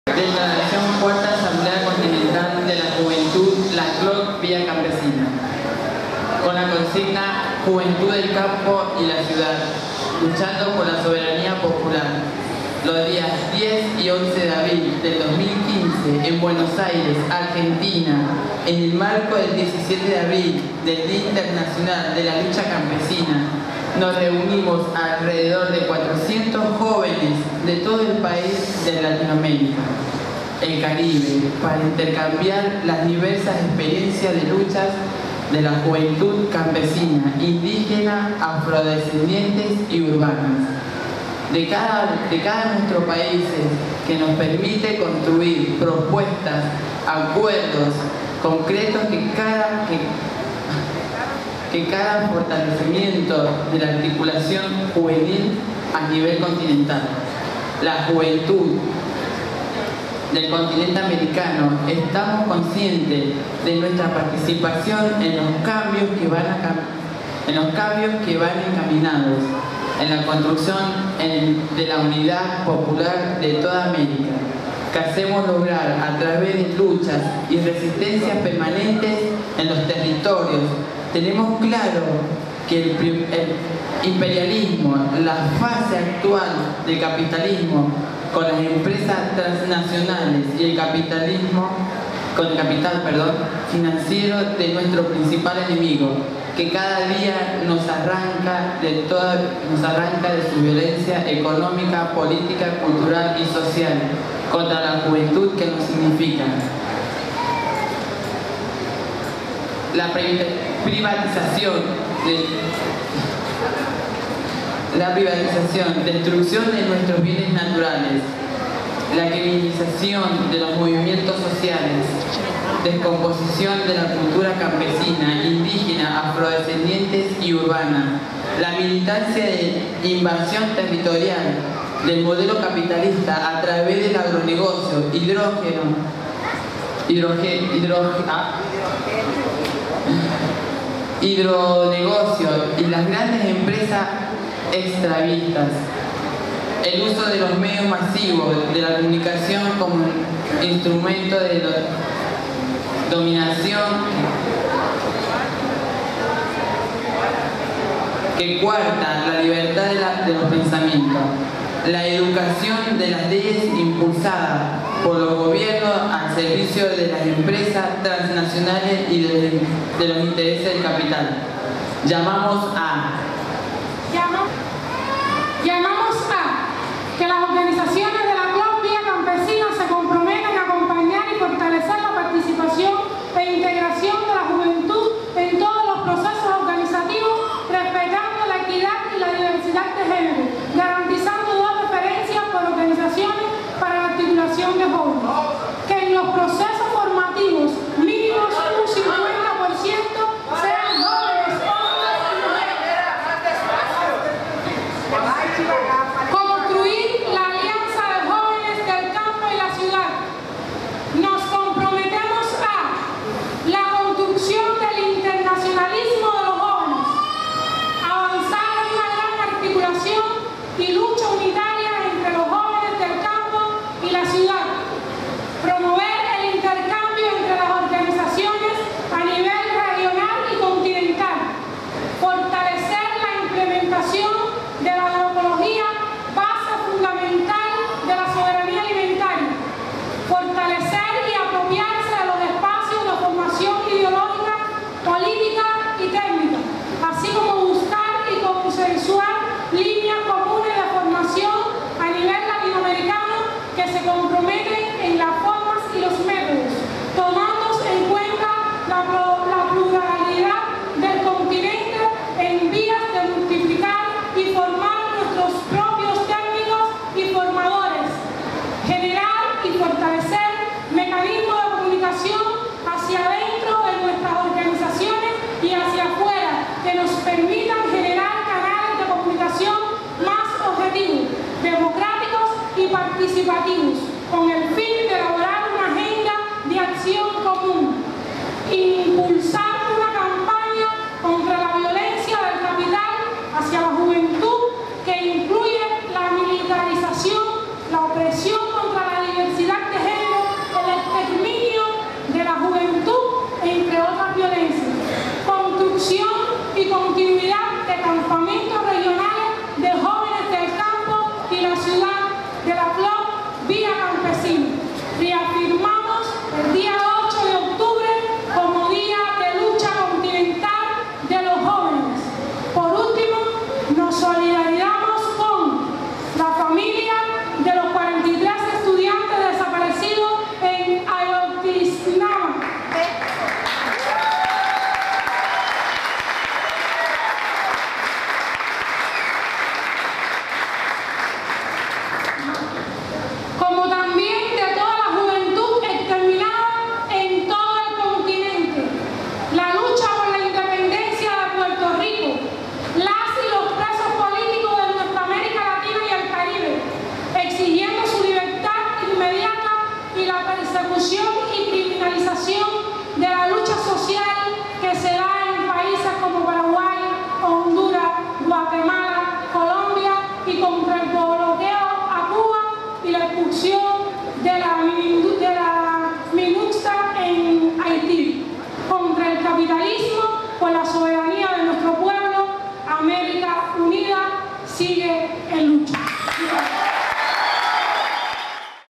Declaración final de la IV Asamblea de la Juventud CLOC Vía Campesina - video
Con la consigna "Juventud del campo y la ciudad, Luchando por la soberanía popular", los días 10 y 11 de Abril del 2015 en Buenos Aires, Argentina, en el marco del 17 de Abril, día internacional de la lucha campesina, nos reunimos alrededor de 400 jóvenes de todos los países de América latina y el Caribe para intercambiar las diversas experiencias de lucha de la juventud campesina, indígena, afrodescendientes y urbanas de cada uno de nuestros países, que nos permitieron construir propuestas y acuerdos concretos de cara al fortalecimiento de la articulación juvenil a nivel Continental.